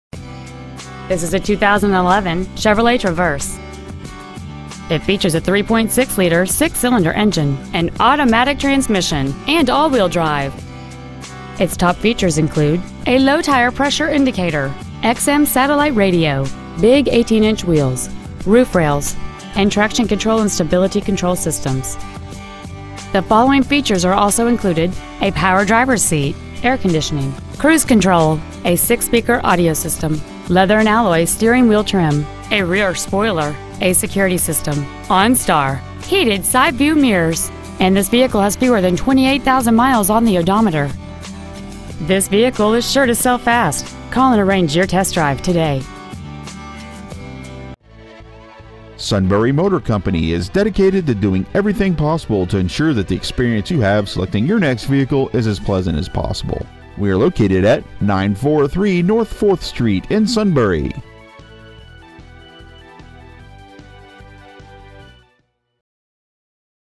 2011 CHEVROLET TRAVERSE Sunbury PA
Year : 2011
 Make : CHEVROLET
 Model : TRAVERSE
 Engine : 3.6L V6 24V GDI DOHC
 Trans . : 6-SPEED AUTOMATIC
 Exterior : SILVER
 Miles : 27,912

 Stock : HH977A